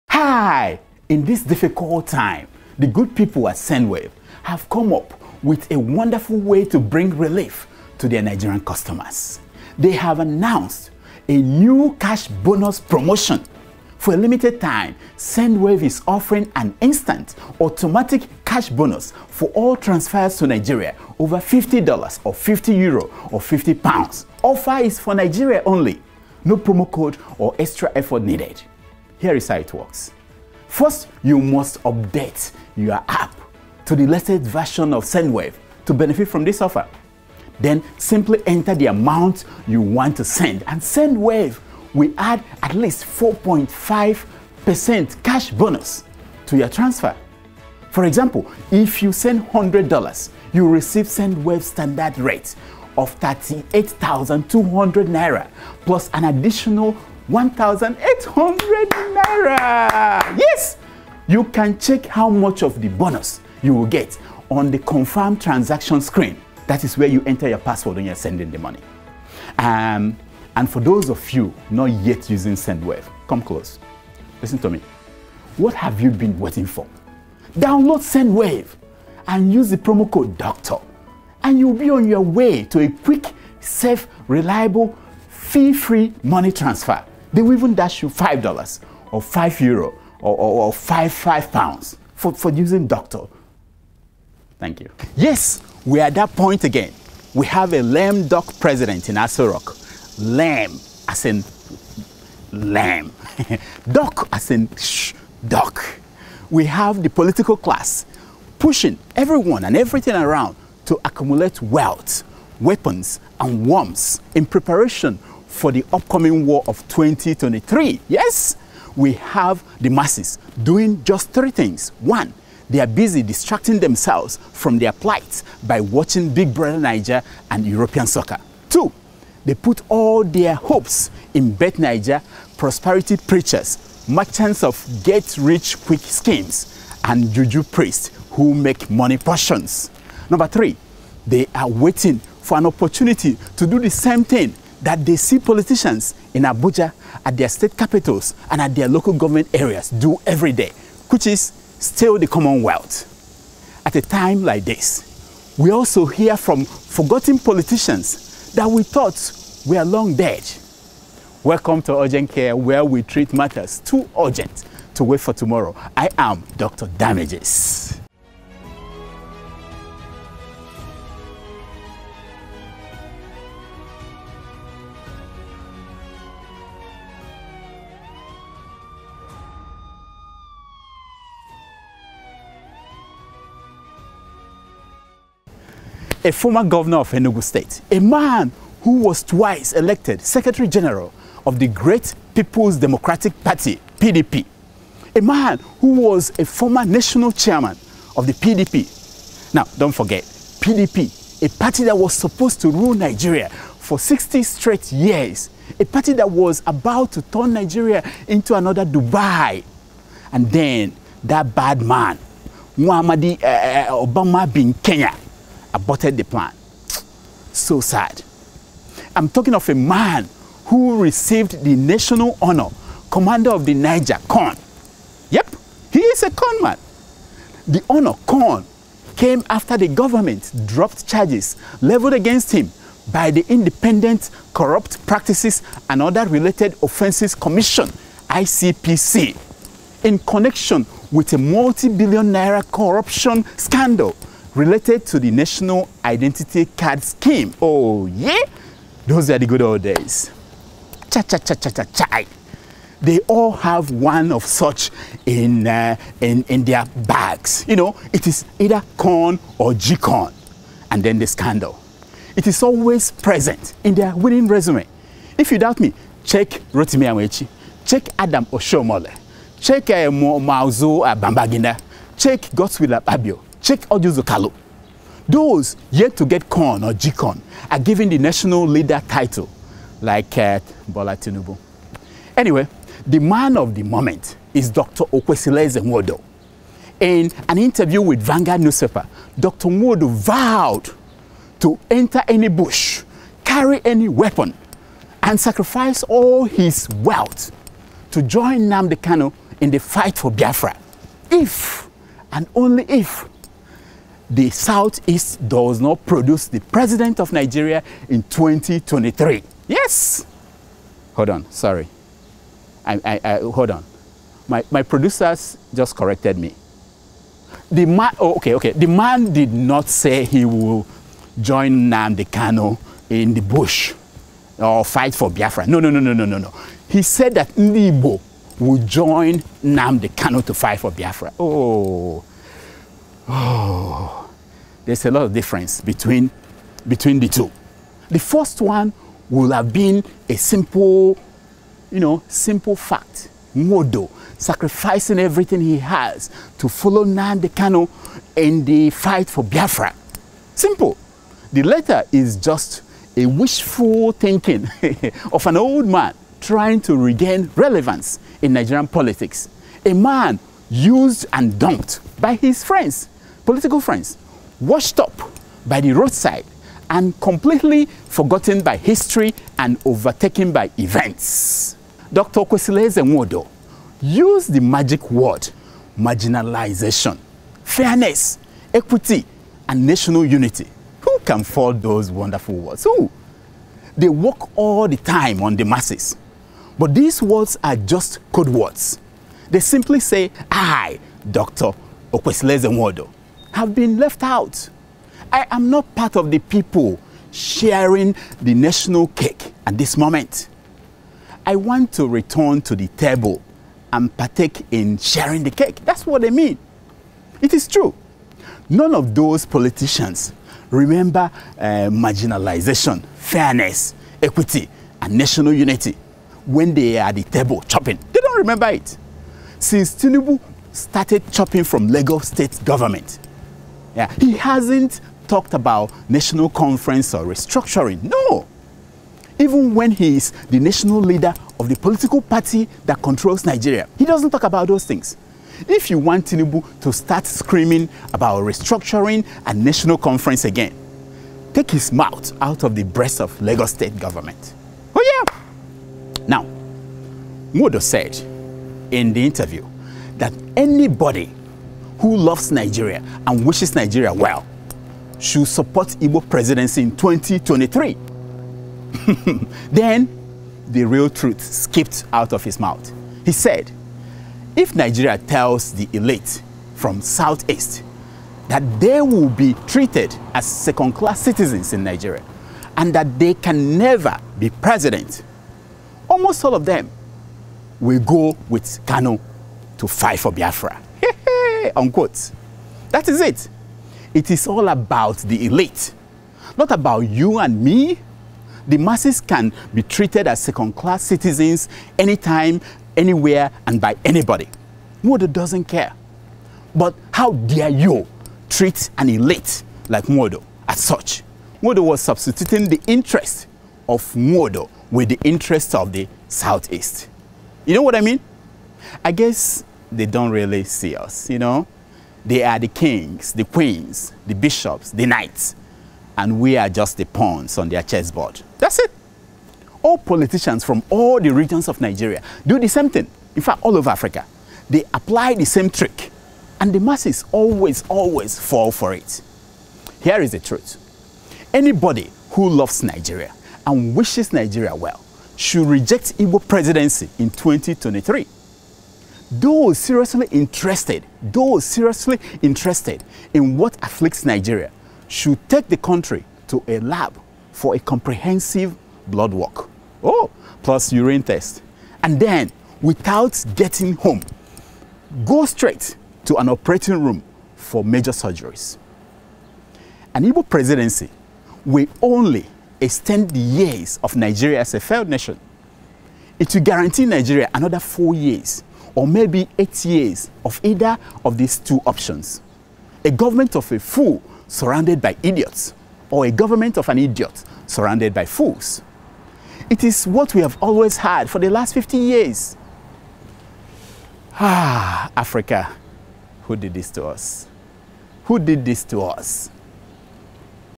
Urgent Care 13: Why you must reject Igbo Presidency in 2023. Let Nwodo join the fight for Biafra
In an interview with Vanguard newspapers, Dr. Okwesilieze Nwodo, former Governor of Enugu State and former Chairman of the People’s Democratic Party (PDP), warned that Ndigbo would join Nnamdi Kanu in the fight for Biafra if Nigeria does not concede the presidency in 2023 to the Igbo. Dr. Damages diagnoses Dr. Okwesilieze Nwodo and explains why those who wish Nigeria well must reject the Igbo presidency in 2023. Your good doctor articulates the only two things Igbo presidency will give Nigeria. As a public service, he goes on to explain what Nigeria really really needs in 2023.